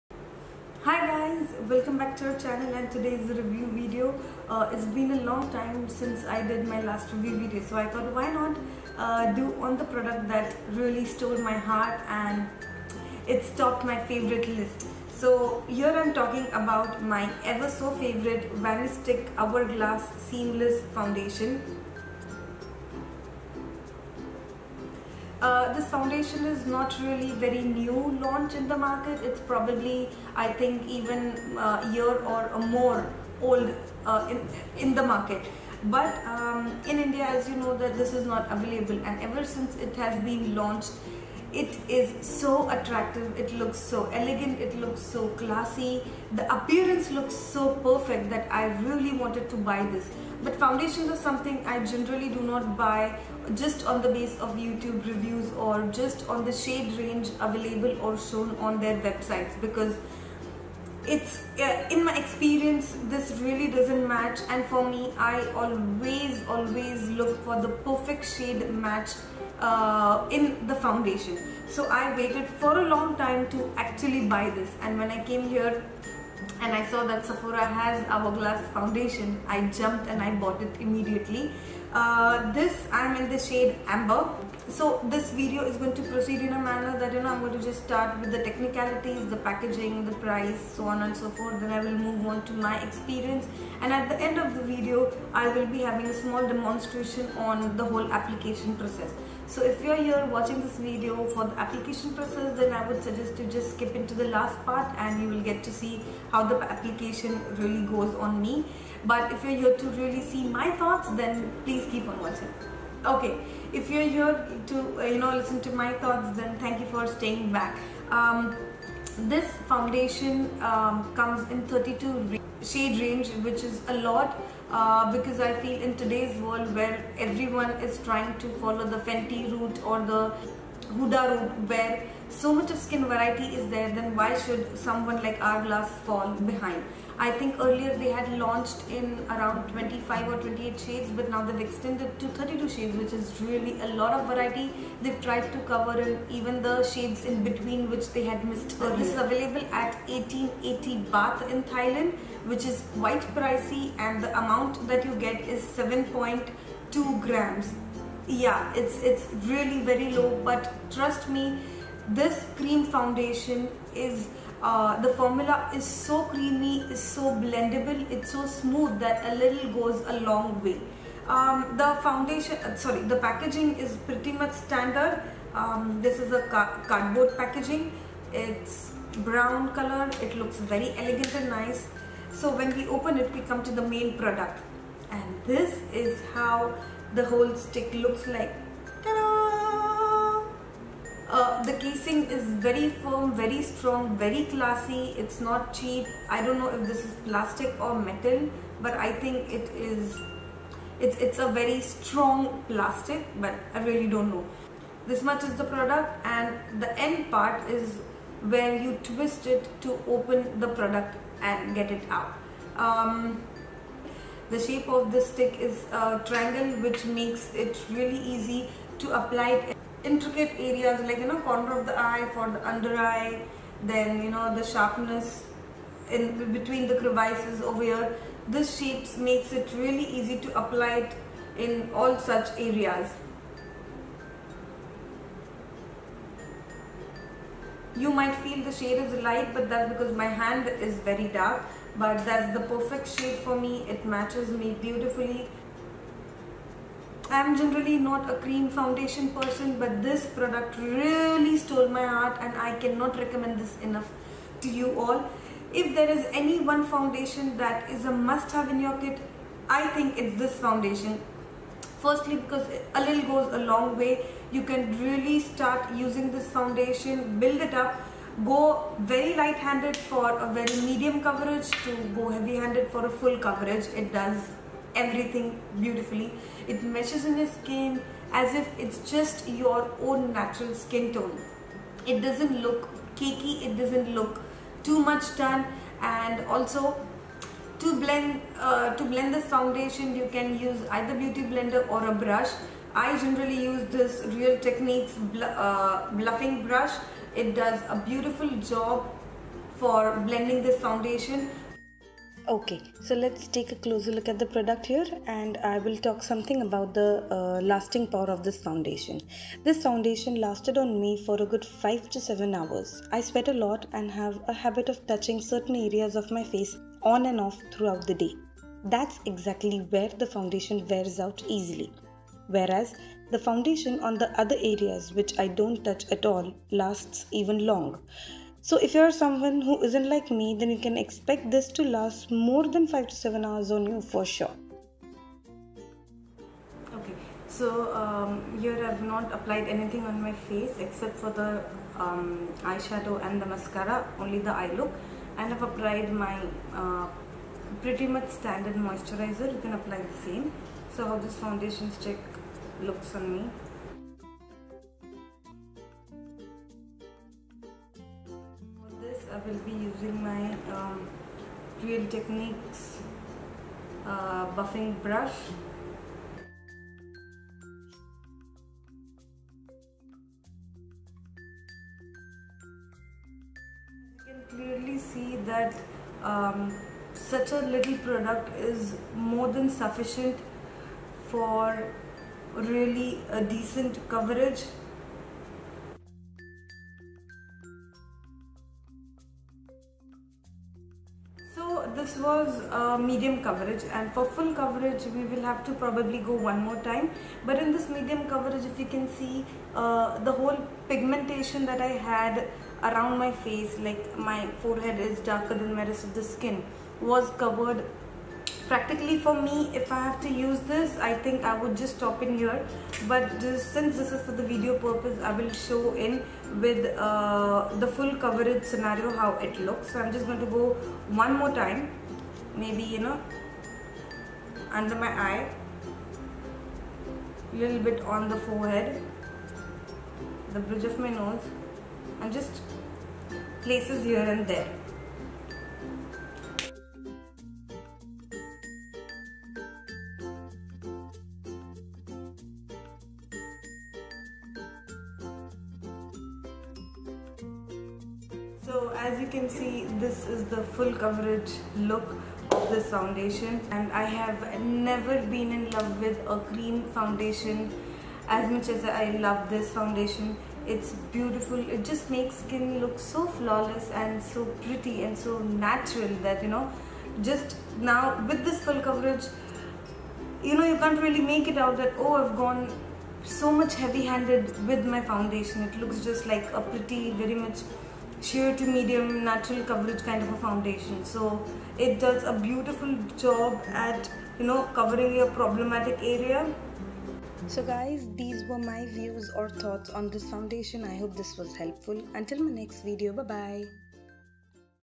Hourglass Vanish Stick Foundation Review l FOUNDATION FOR BROWN SKIN l NC 43.5 l Ajita
Hello all,

Today is a Review Video and this is a review on Hourglass Vanish Stick Foundation..

Until our next Video lots of Love and Stay happy. !!!

Dont forget to tag your pics with beautywithatwist.as

Love
xoxoxoxo